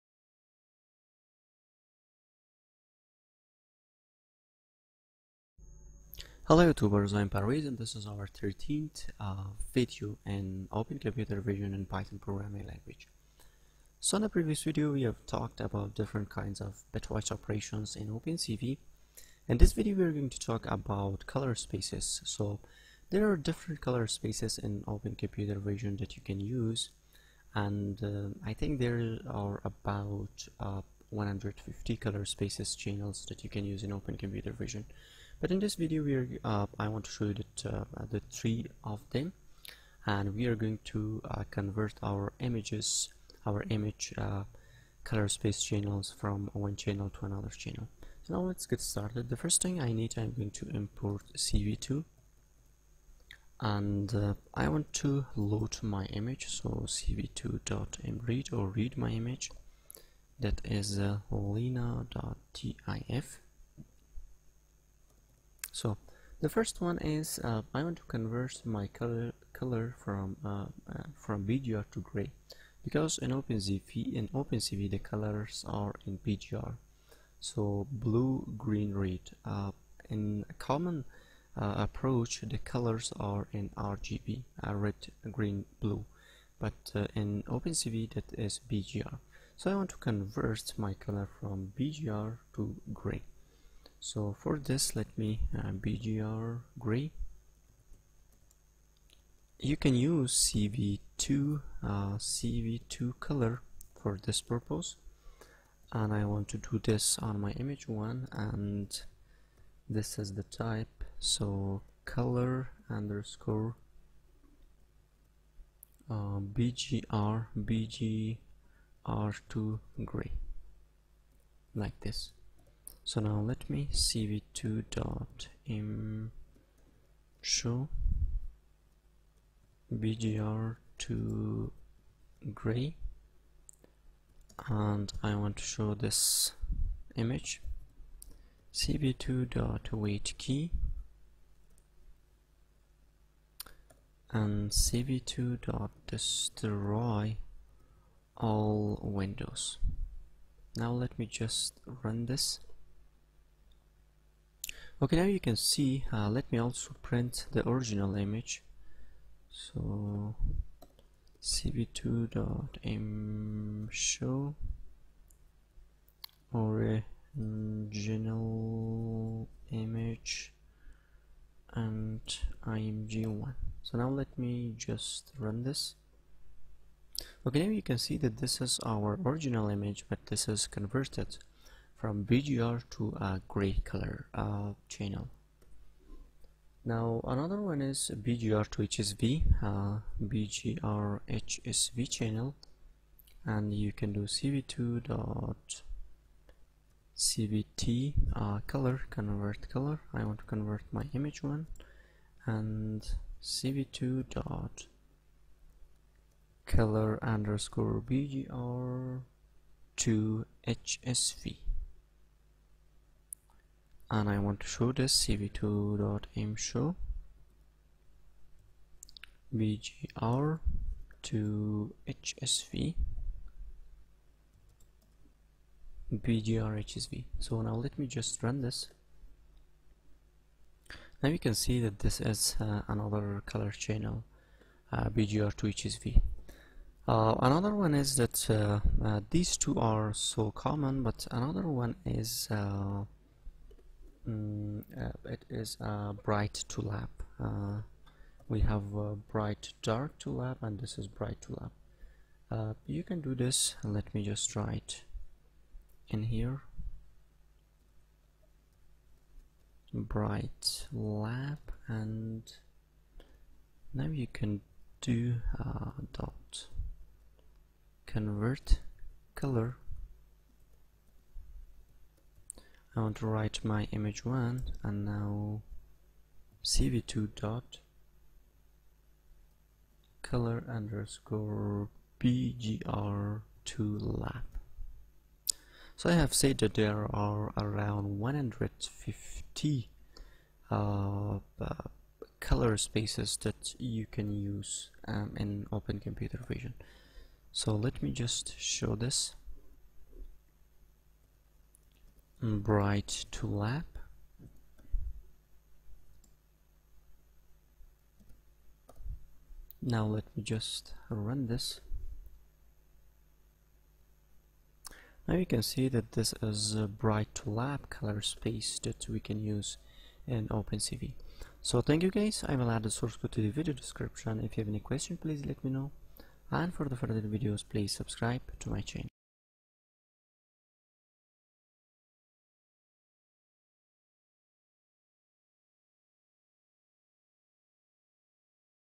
OpenCV Python Color Spaces
In this OpenCV Python video we are going to talk about Color Spaces Introduction.

There are more than 150 color-space conversion methods available in OpenCV. But we will look into only two which are most widely used ones, BGR ↔ Gray and BGR ↔ HSV.

For color conversion, we use the function cv.cvtColor(input_image, flag) where flag determines the type of conversion.

For BGR → Gray conversion we use the flags cv.COLOR_BGR2GRAY. Similarly for BGR → HSV, we use the flag cv.COLOR_BGR2HSV. To get other flags.